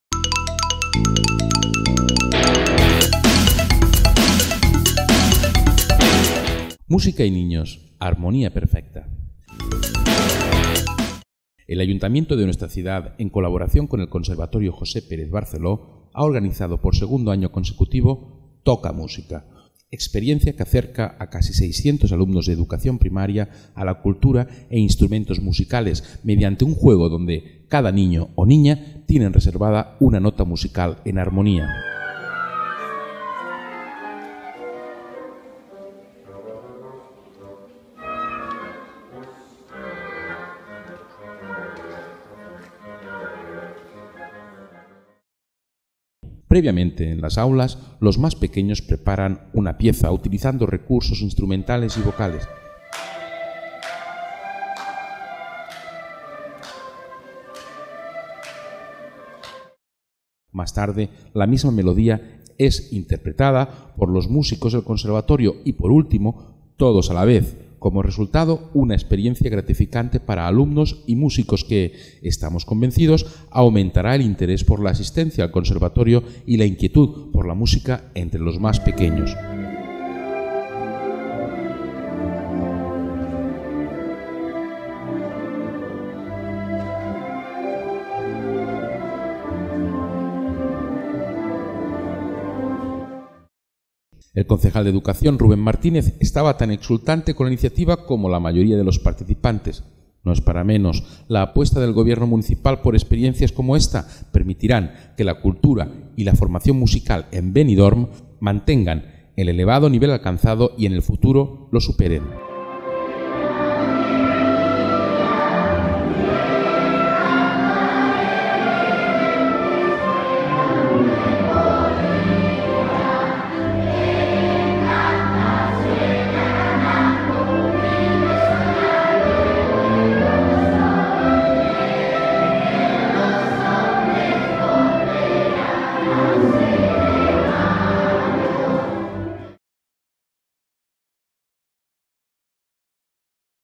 Música y niños, armonía perfecta
El Ayuntamiento de nuestra ciudad en colaboración con el Conservatorio 'José Pérez Barceló' ha organizado por segundo año consecutivo 'Toca Música'.